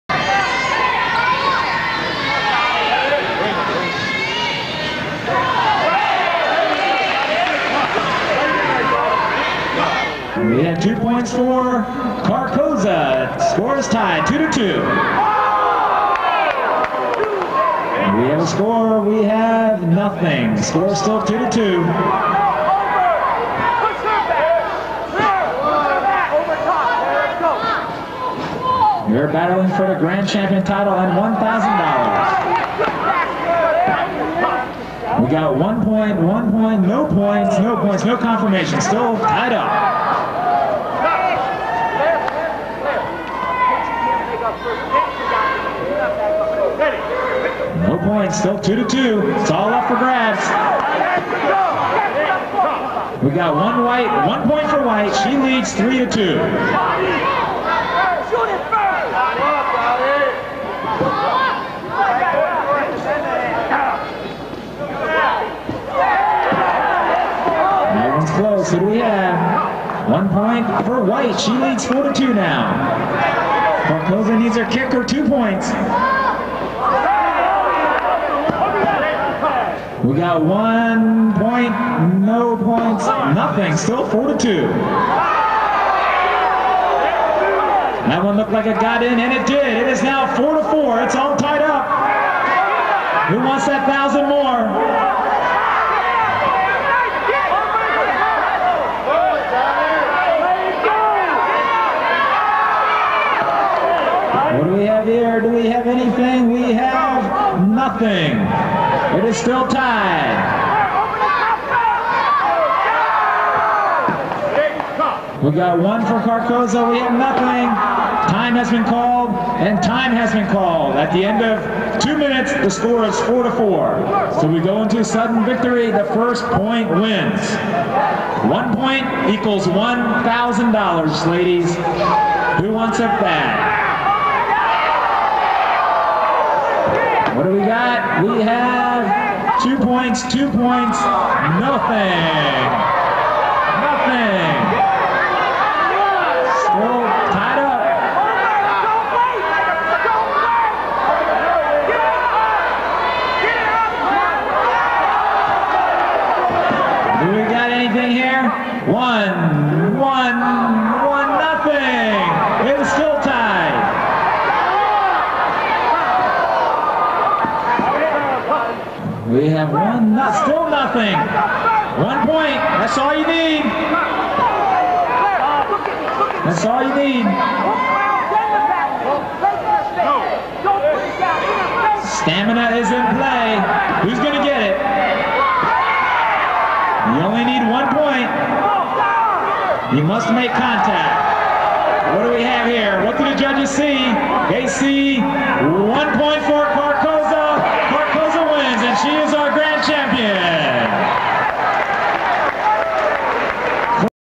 Claire Cocozza vs Dottie White 2001 Capitol Classics Karate Tournament
Claire Cocozza vs Dottie White 2001 Capitol Classics Karate Tournament. This video was video taped and edited by myself and my company Black and Blue Video Productions, Inc.